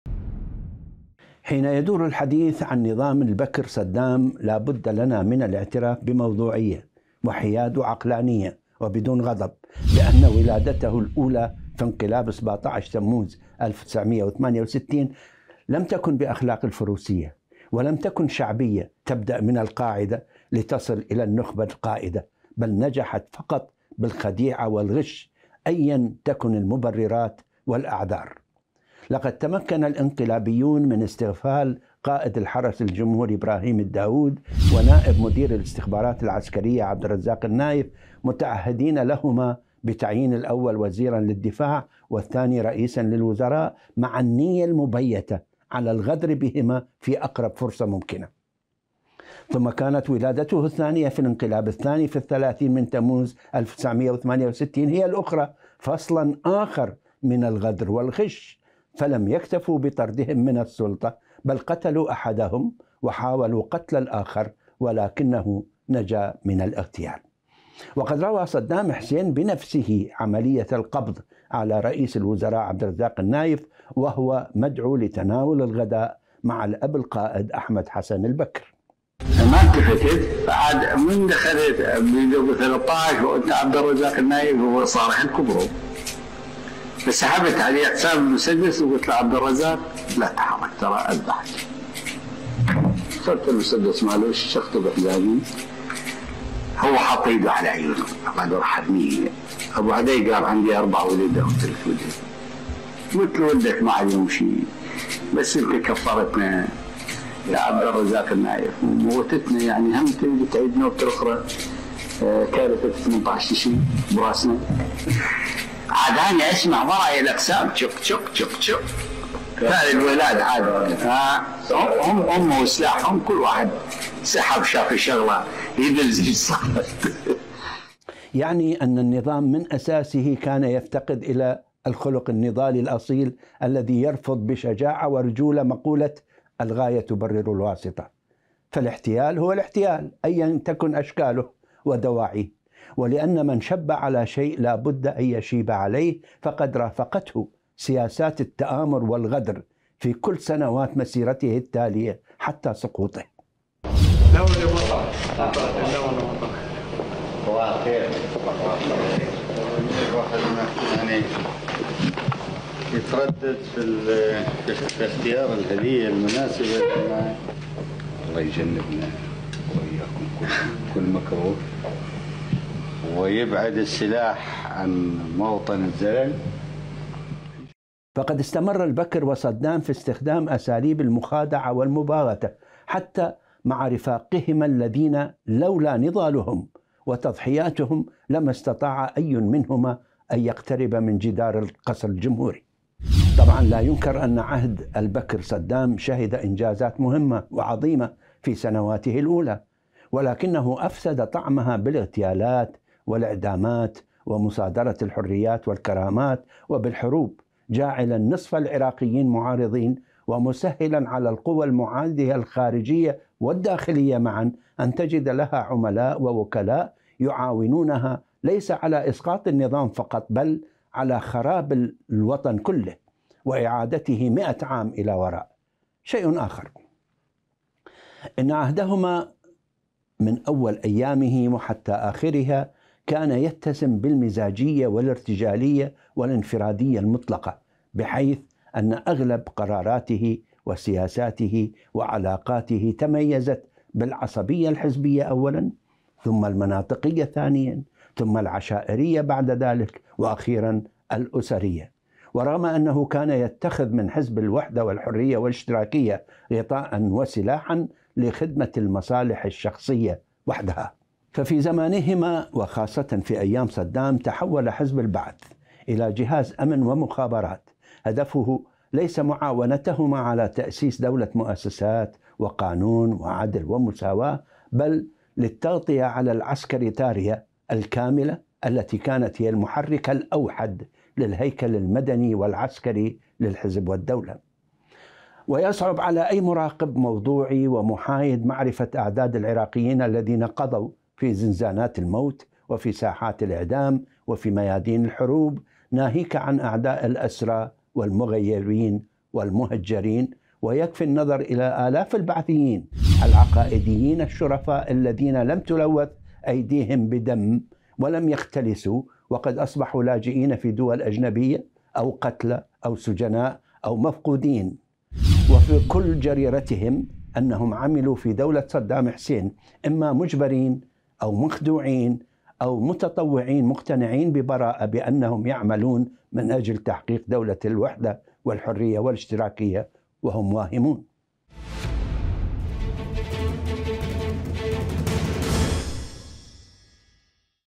تسجيل صوتي نادر لـ صدام حسين .. هكذا سيطر على القصر الجمهوري
موقع قناة التغيير الفضائية  الالكتروني